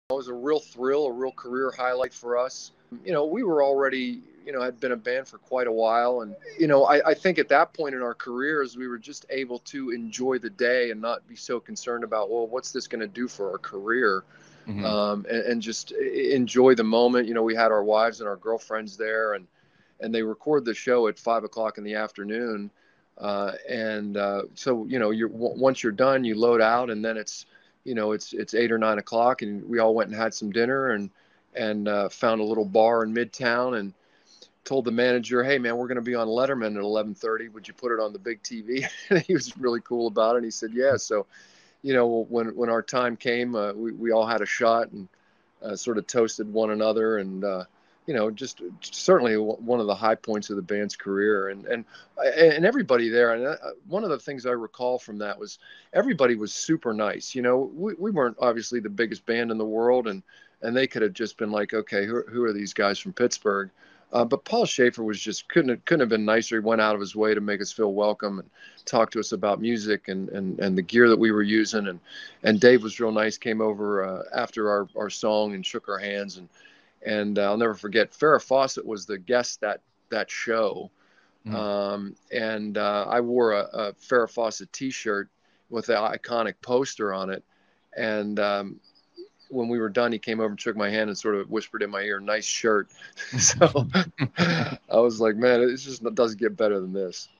The Clarks on playing "The Late Show with David Letterman"
In this Youngstown Studio interview clip, @theclarks frontman @scottblaseymusic talks about the band's experience playing "The Late Show with David Letterman."